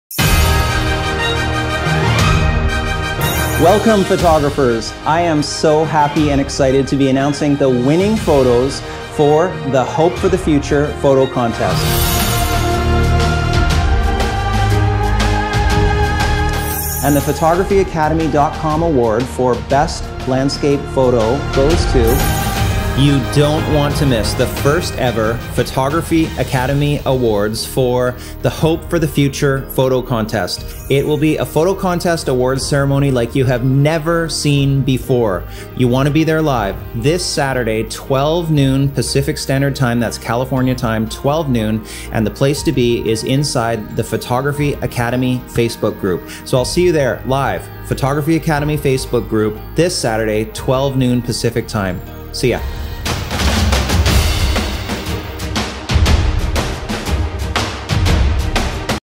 Photo Contest Awards - Watch Them LIVE
Watch the awards LIVE on Saturday, April 18th at 12:00 noon PST.  Join the Photography Academy Facebook group to watch them live

Learn how to take amazing photos with the camera you already own...without boring techno-babble.  Take the free webclass